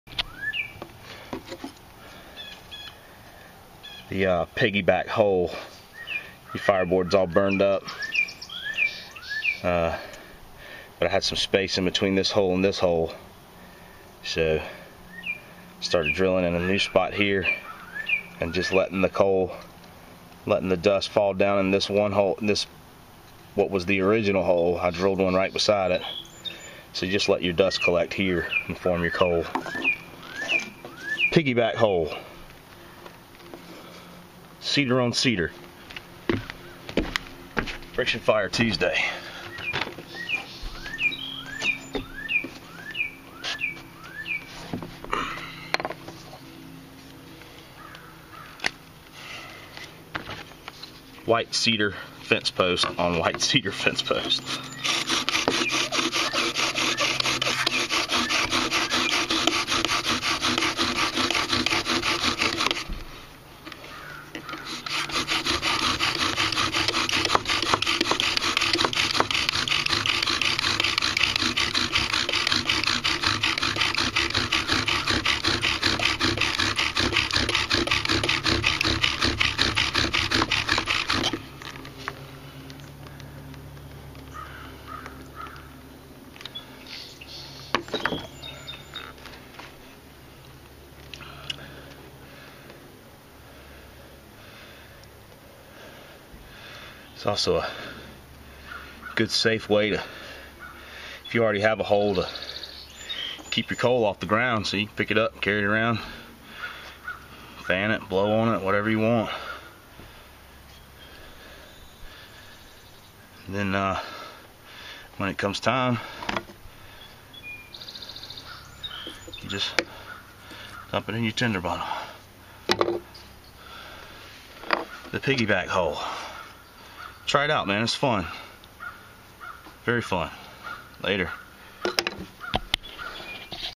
Bowdrill Piggyback!
A great way to get more holes out of your hearth board.